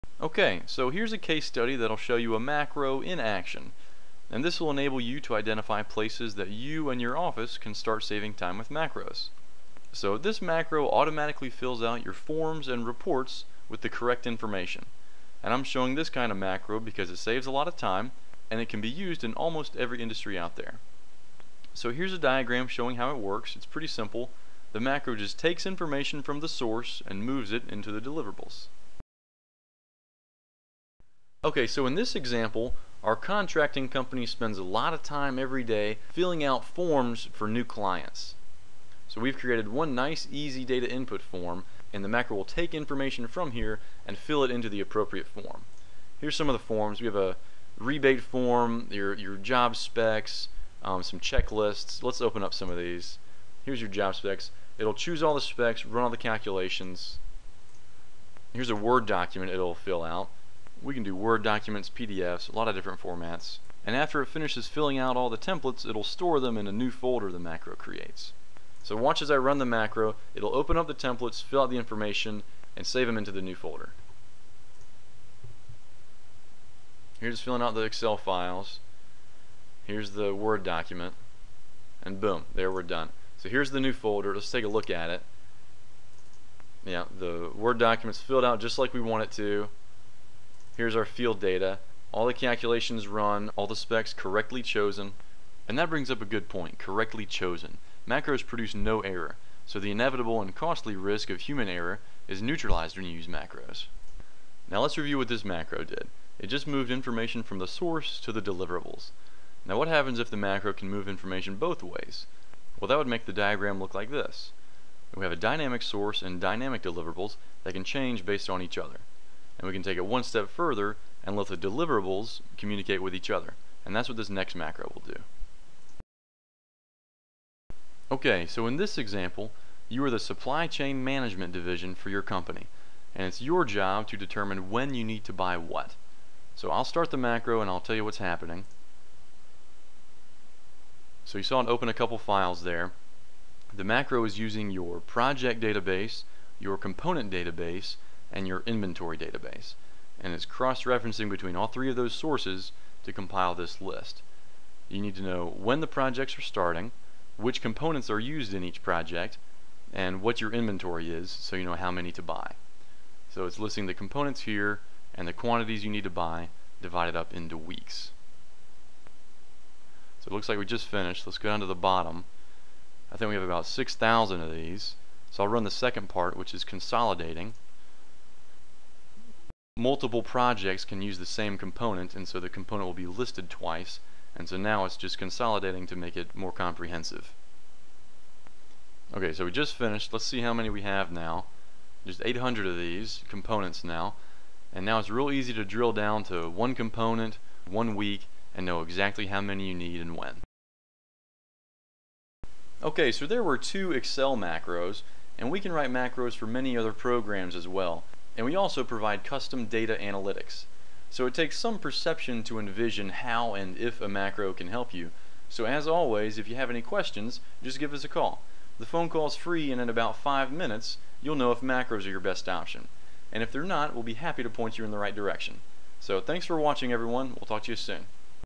Excel Macros - Preparing a batch of templates (advanced mail merge using macros).
Custom Excel workbooks, add-ins, and integrations.  Mongoose Coding was acquired by Docmo in 2019.

Can we help? Let's talk.